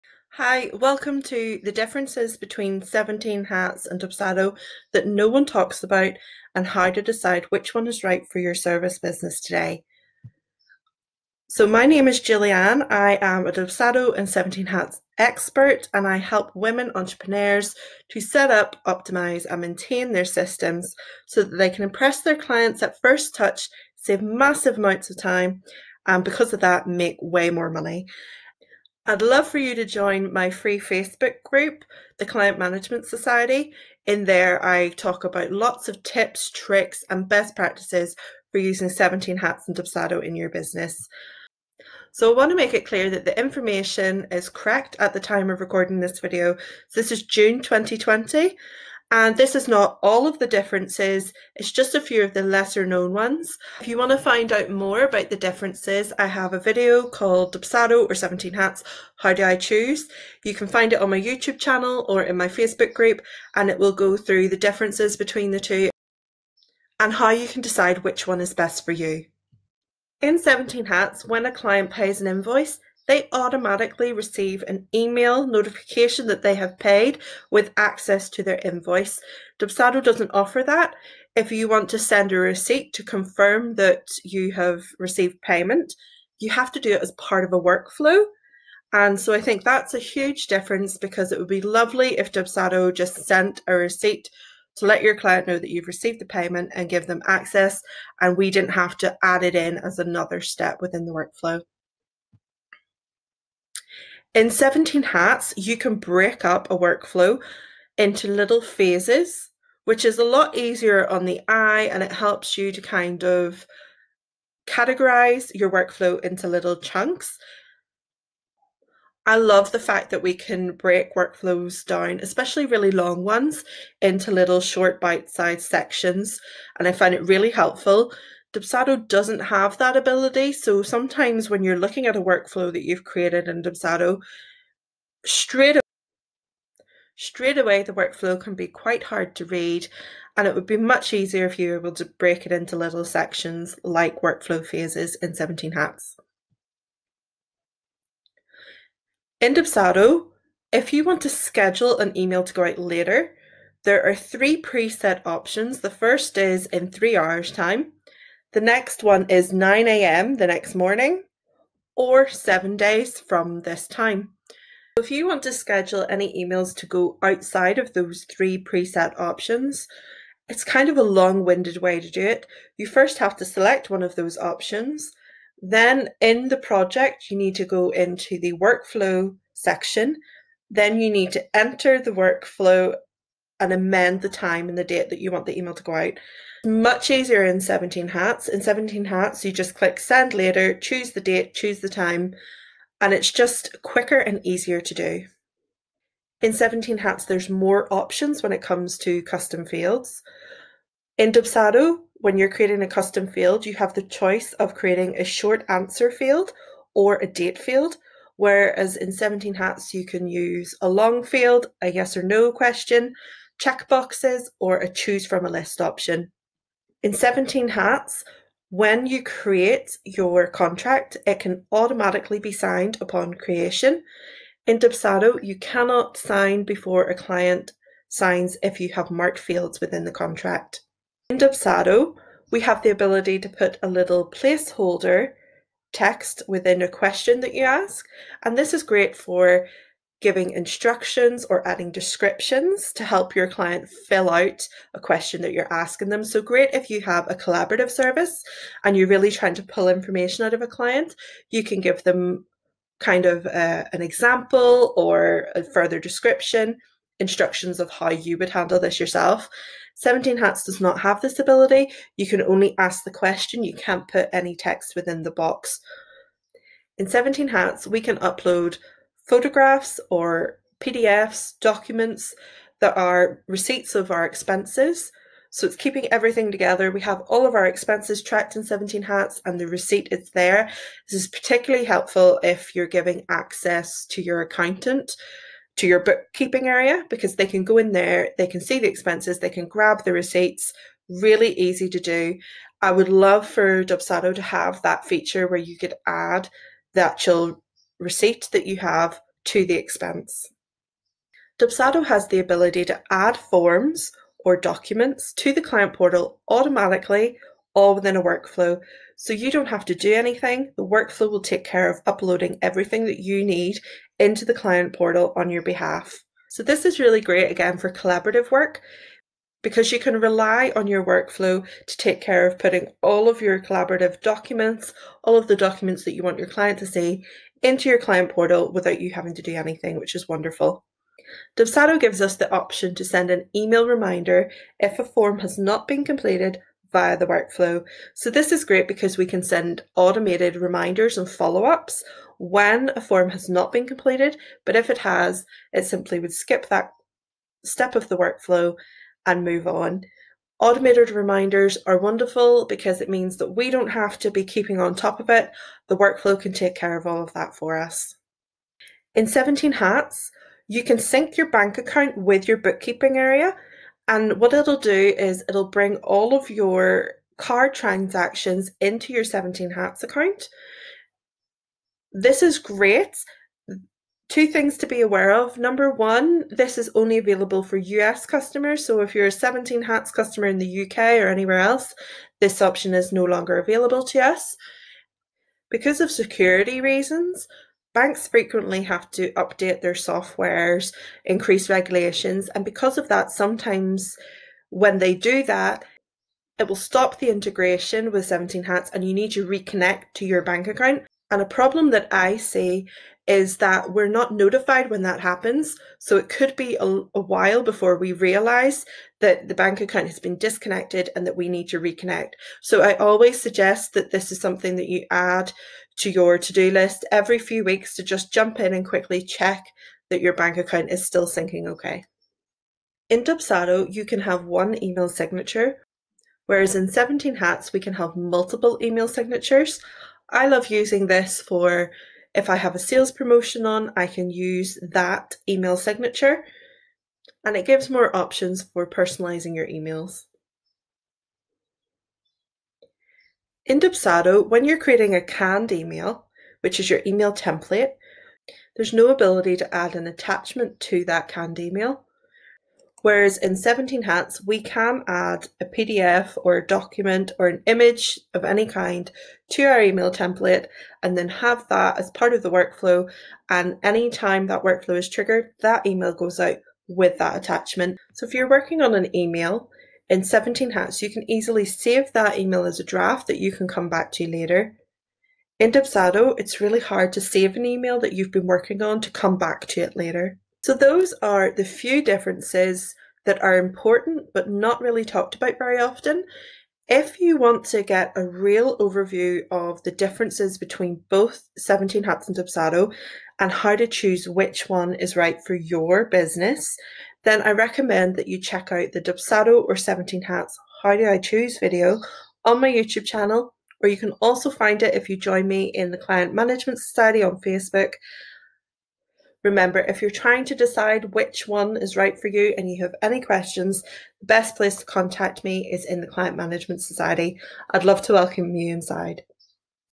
Dubsdo VS 7hats | The differences no one talks about between Dubsado and 17hats
Dubsdo VS 7hats The differences no one talks about.

In this video I cover Dubsado VS 17hats: The Differences No One Talks About And How To Decide Which One Is Right For Your 1:1 Service Business Today!

17hats 30-Day Money Back Guarantee means you can try 17hats for a full month risk free and choose from a range of pricing plans.

WAYS TO WORK WITH ME 1:1:
💻 DONE FOR YOU SETUP & OPTIMISATION SERVICE [Dubsado or 17hats]

💻 // F A V O R I T E B U S I N E S S T O O L S: